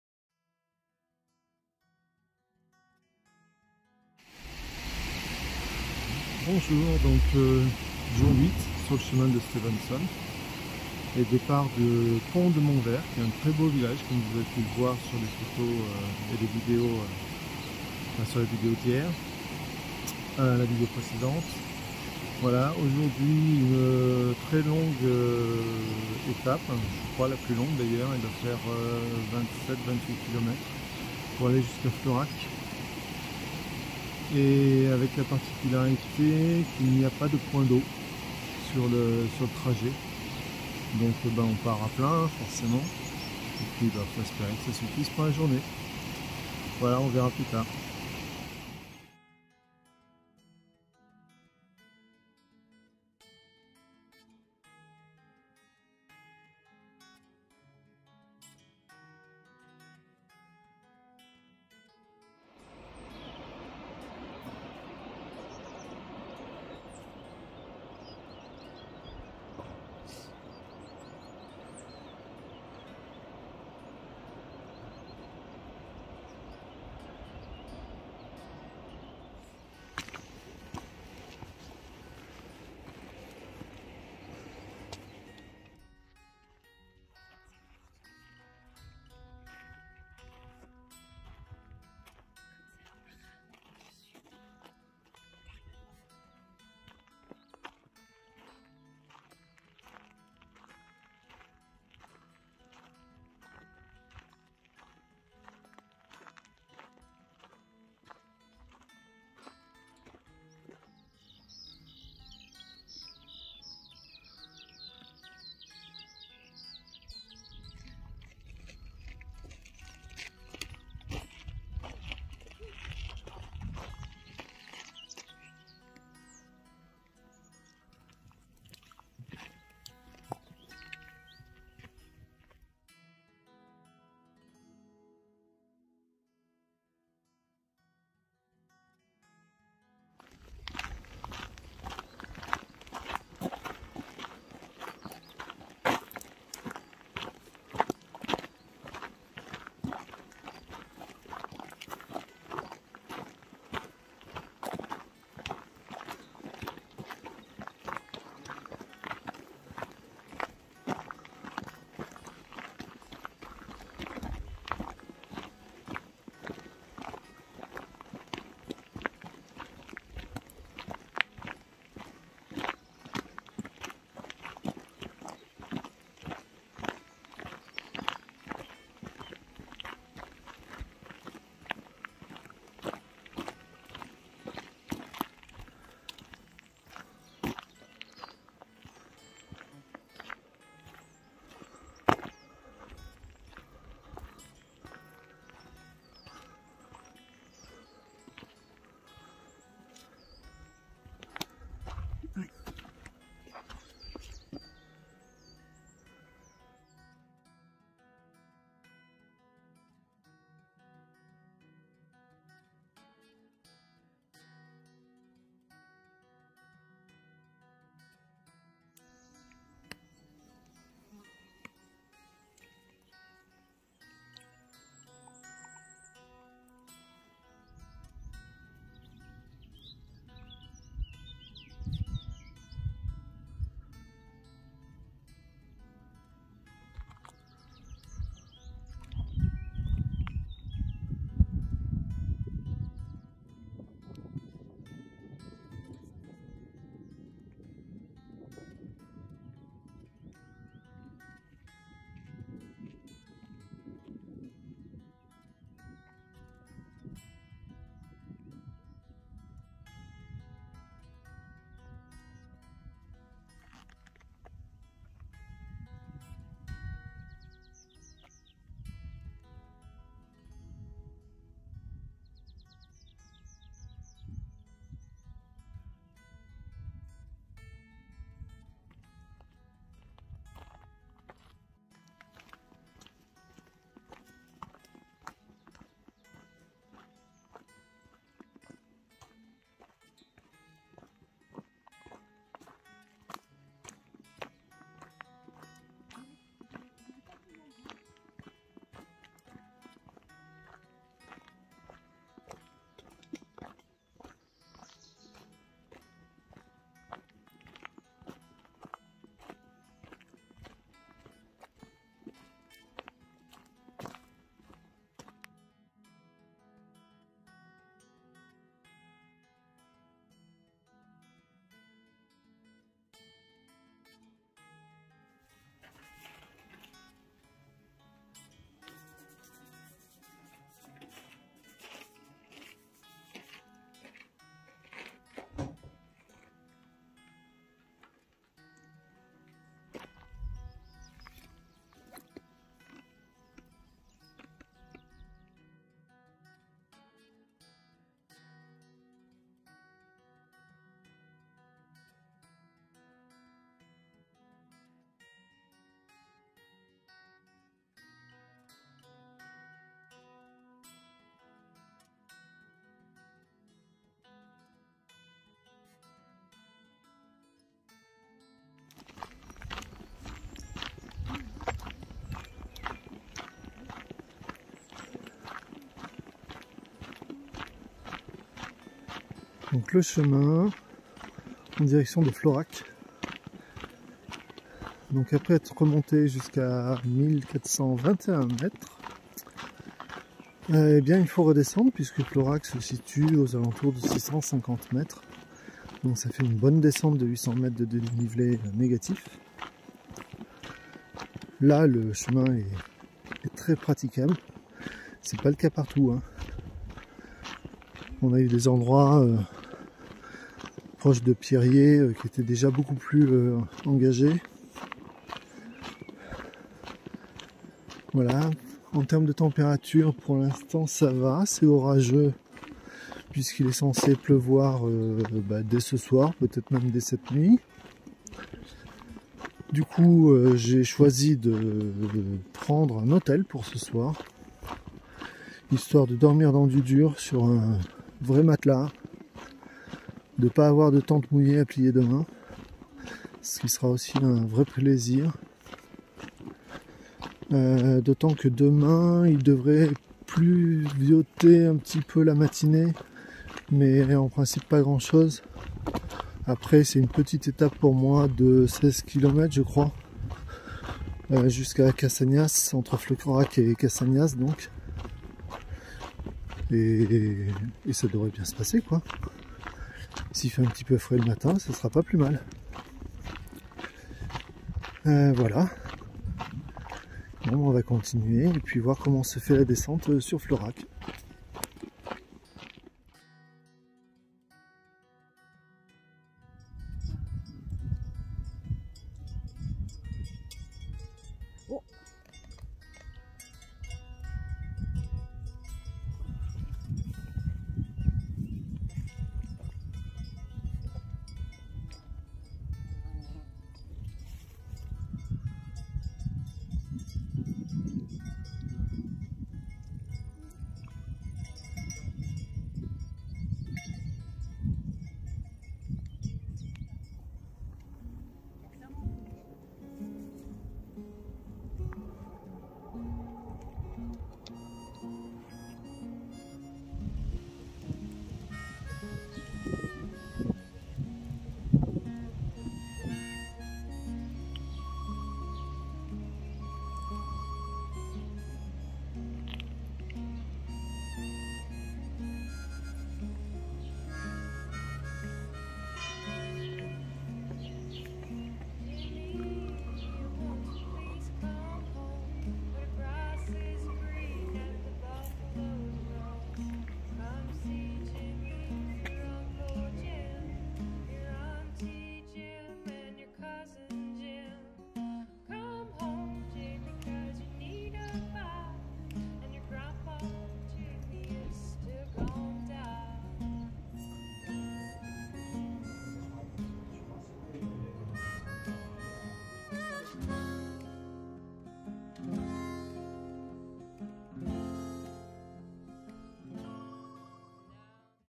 Jour 08 sur le Chemin De Stevenson (GR70) : De Pont-de-Montvert à Florac.
Hôtel du Pont Neuf, à Florac.

Chapitres :
00:00 Intro
00:05 Pont-de-Montvert
02:54 Champlong
03:50 Signal du Bougès
05:27 Cabane de Bonnal
08:41 Florac
09:40 Outro

La trace GPX :
À venir...

Musicien: Moriarty